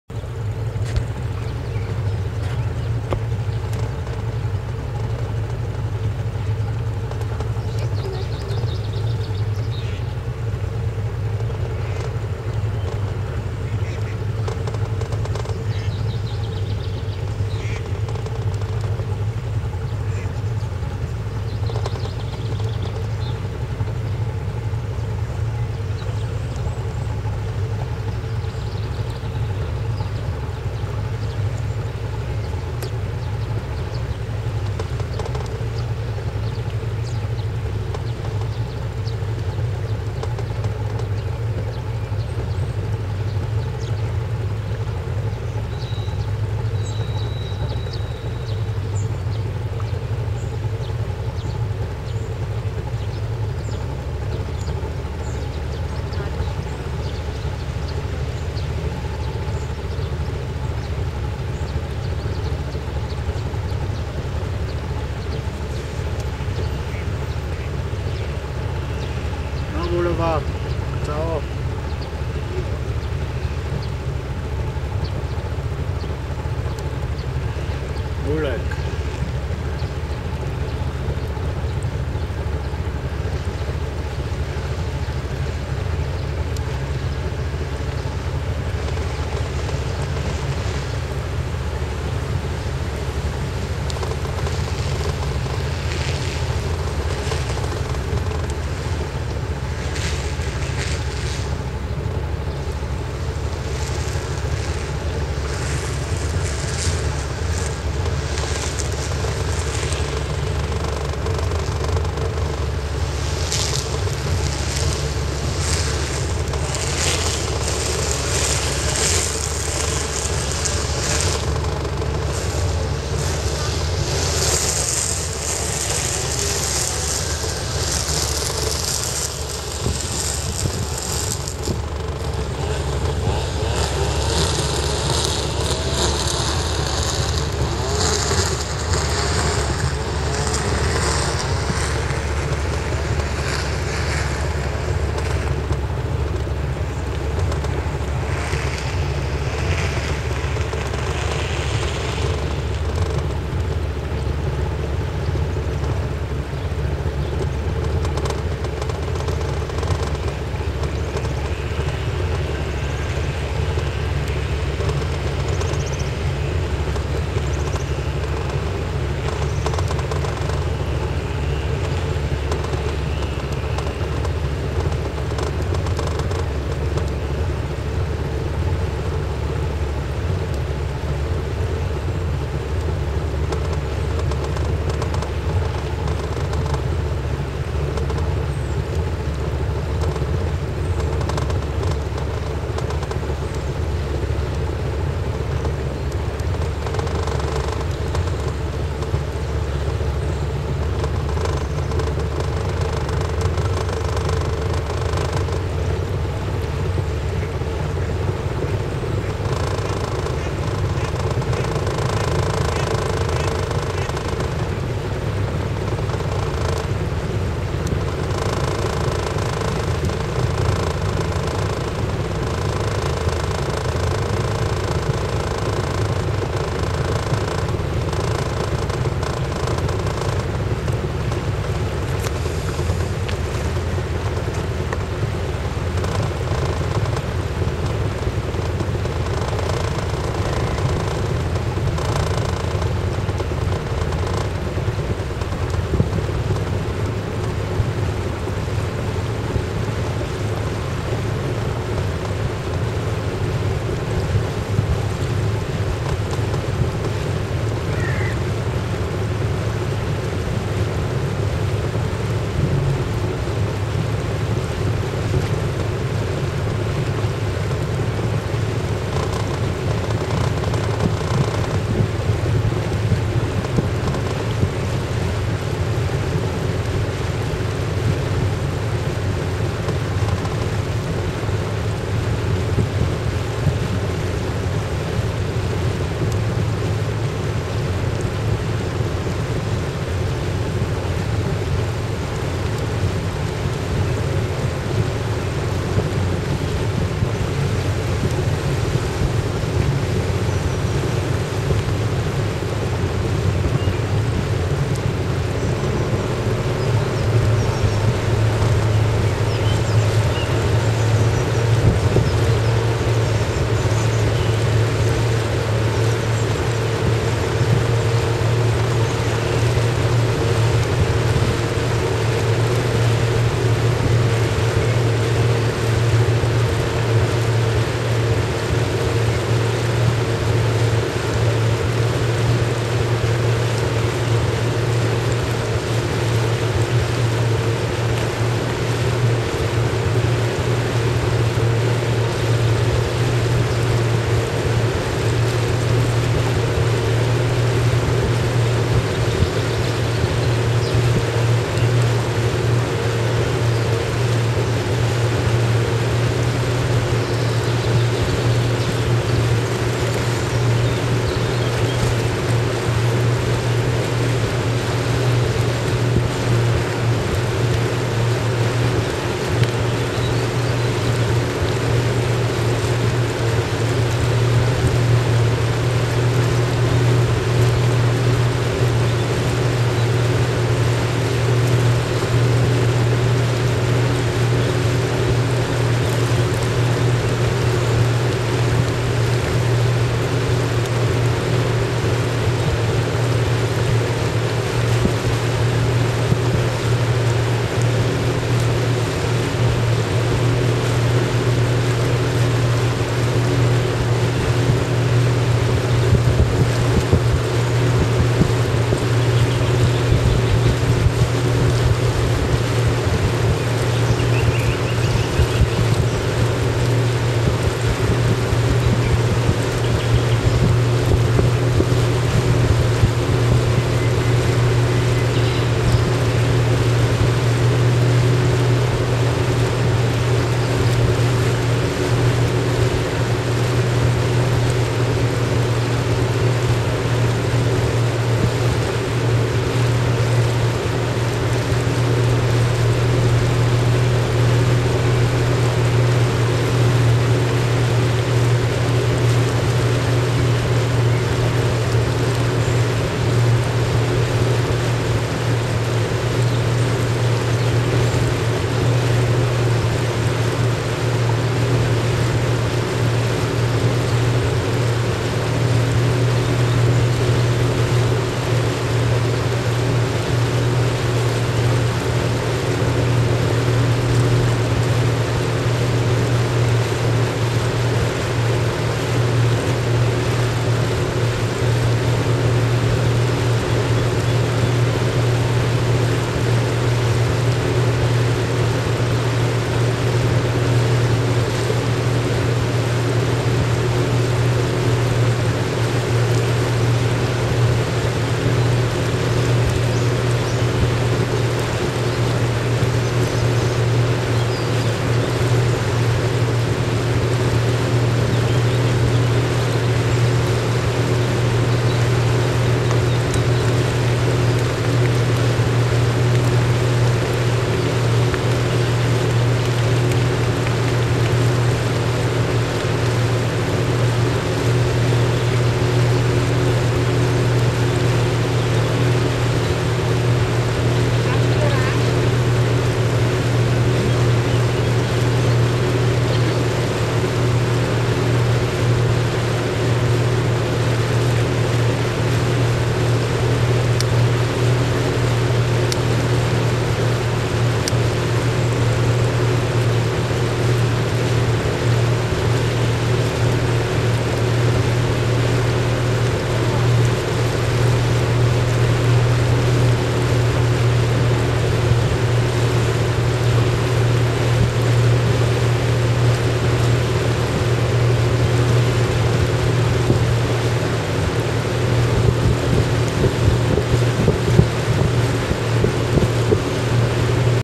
gyoer_mosoni_duna.MP4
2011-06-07 Ungarn, Györ, Mosoni Duna zu Tal
Sound: YAMAHA 40PS 2Takt